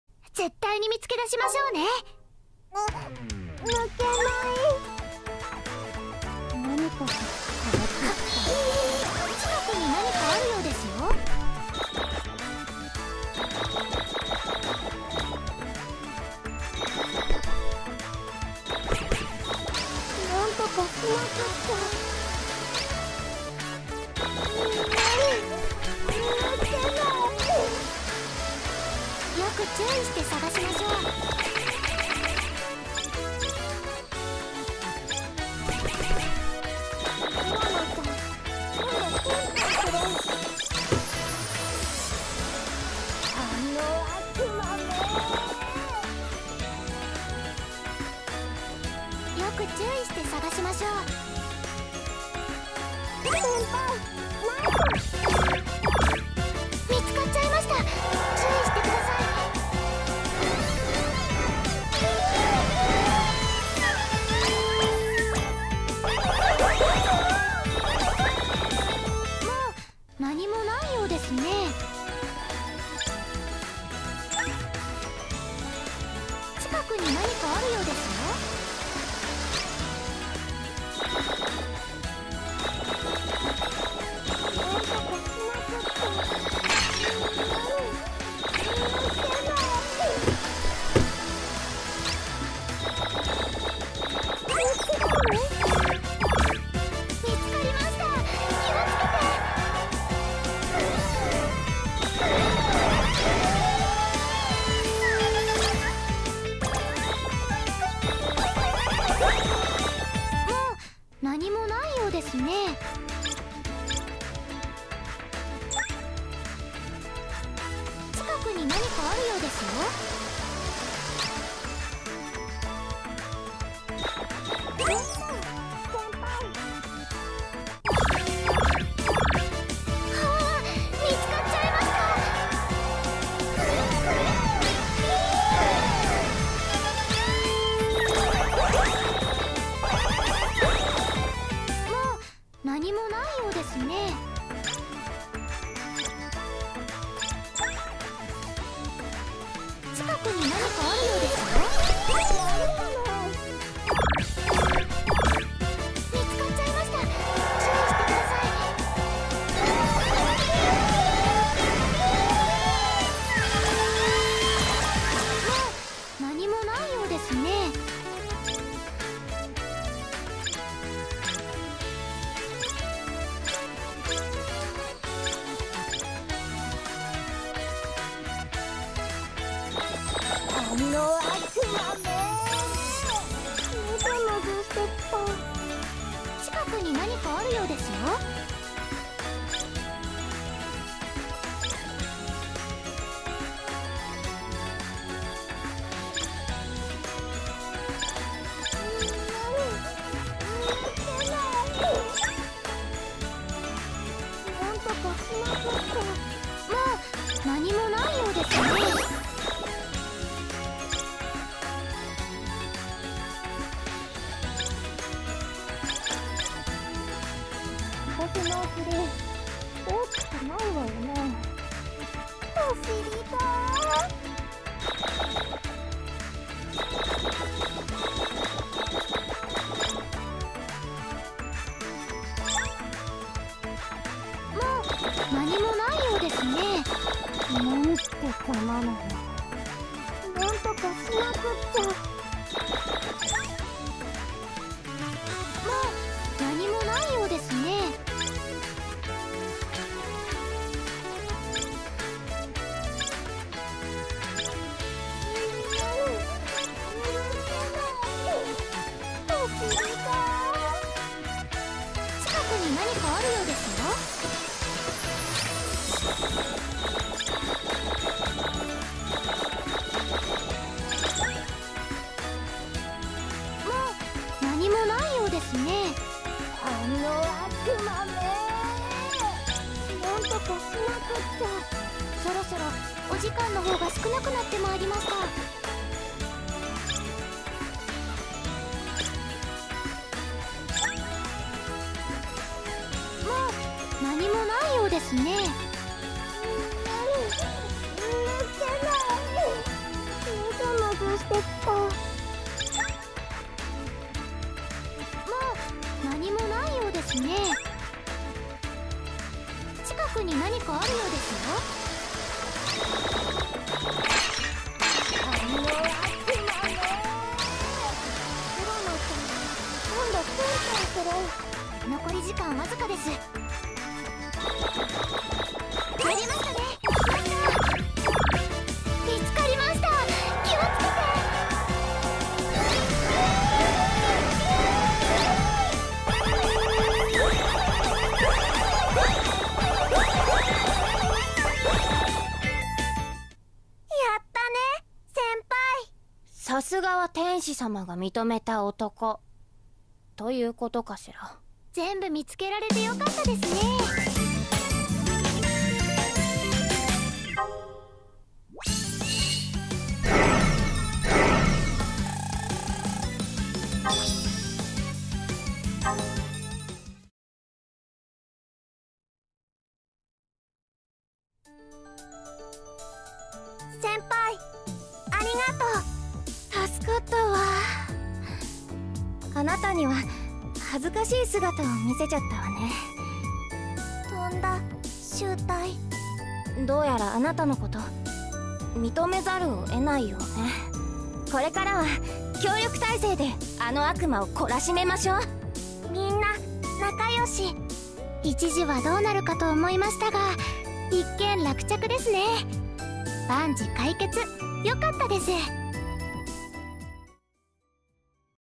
Find The Demons!
Shinobu & Maya have found themselves in a bit of a predicament!